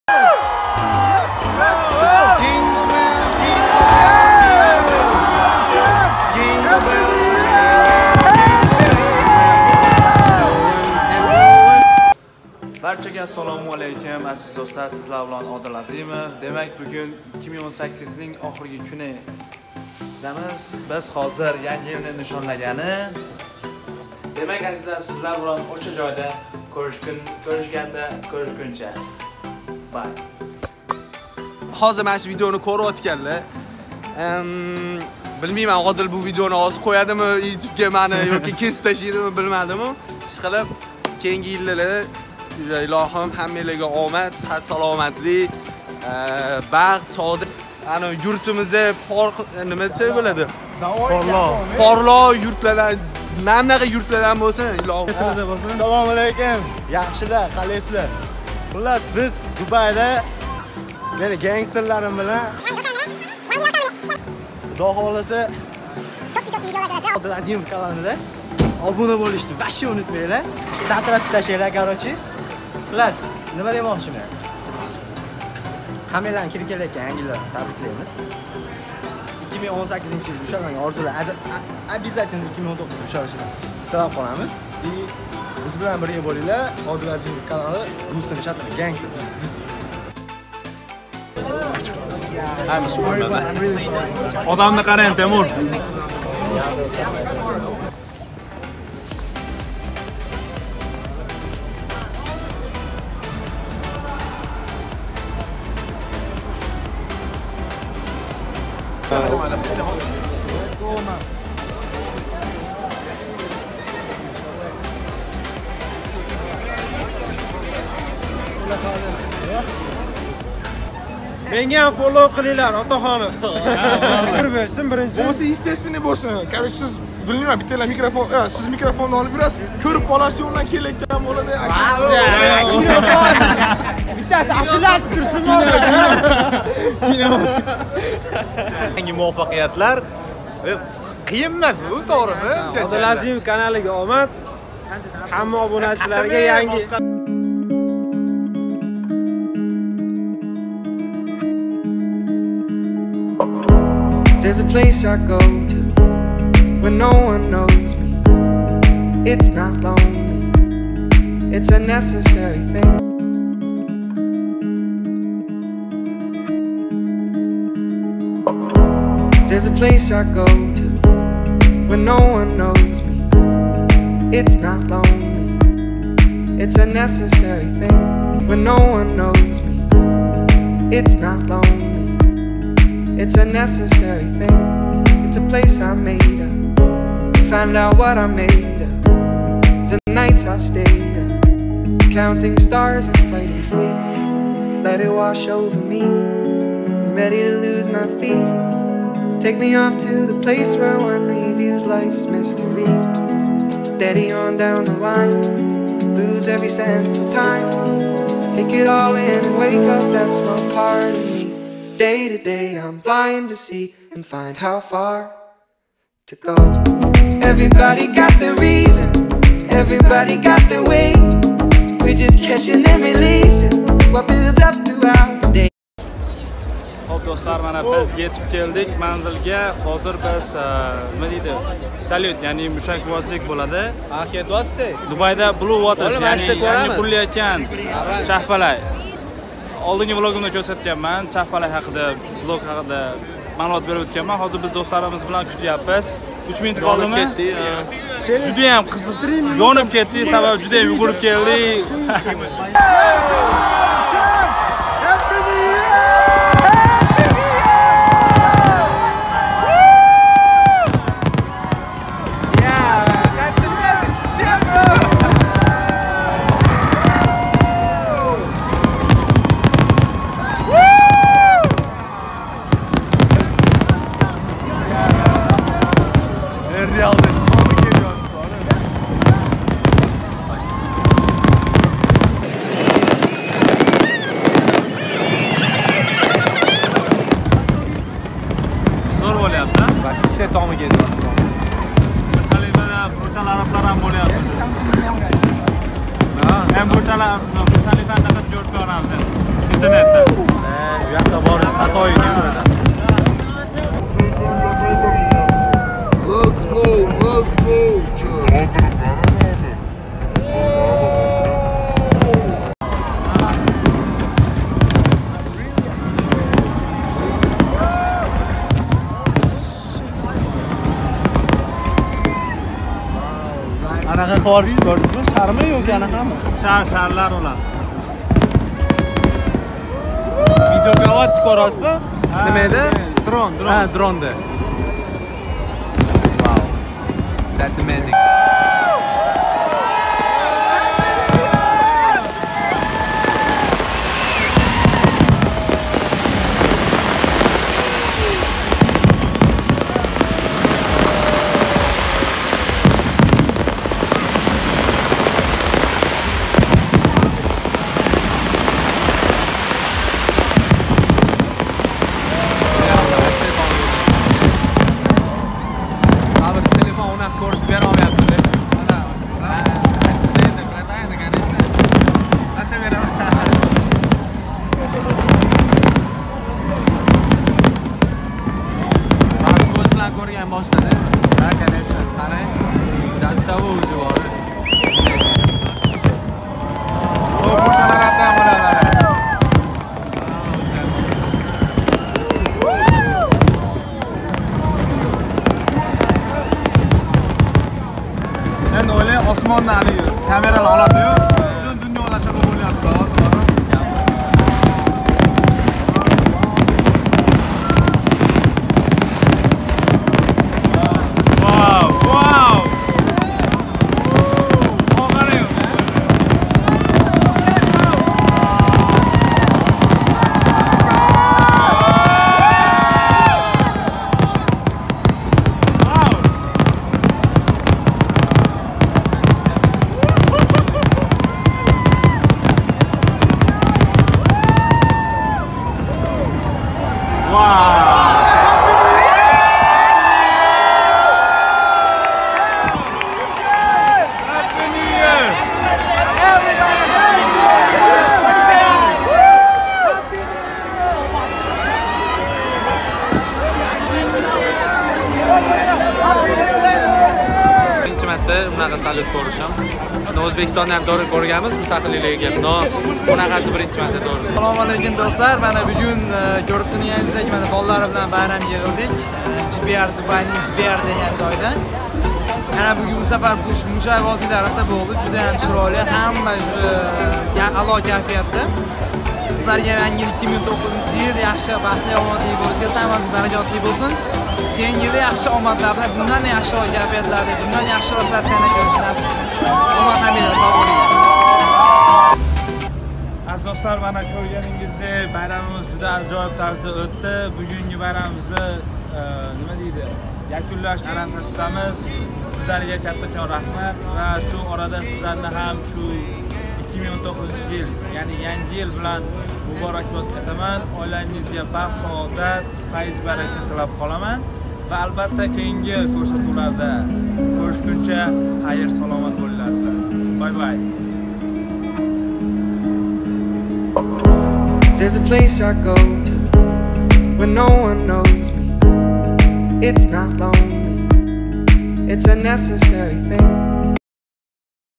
DUBAYDA ANDIJONLIKLAR BIRGALIKDA 2019 YILNI KUTIB OLDIK!
Do'stlarim Kanalimga Obuna Bo'ling!